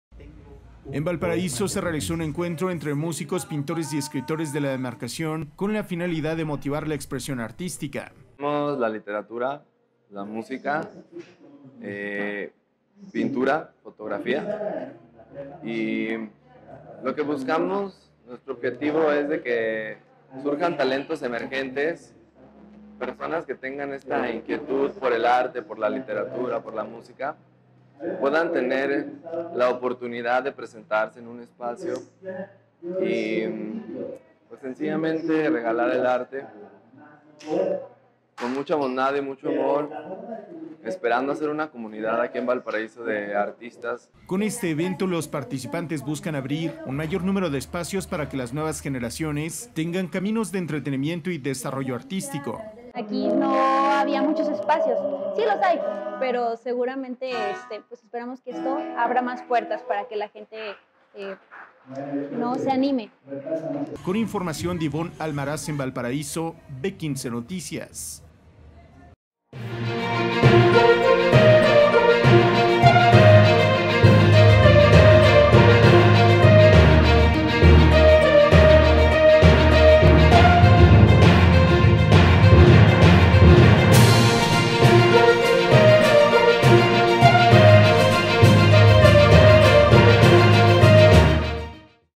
Buscan incentivar el desarrollo de las artes en Valparaíso
Valparaíso, Zacatecas.- En Valparaíso se realizó un encuentro entre músicos, pintores y escritores de la demarcación con la finalidad de motivar la expresión artística.

Con este evento, los participantes buscan abrir un mayor número de espacios para que las nuevas generaciones tengan caminos de entretenimiento y desarrollo artístico.

Con información de Ivoón Almaraz, en Valparaíso/Omar Hernández… B-15 noticias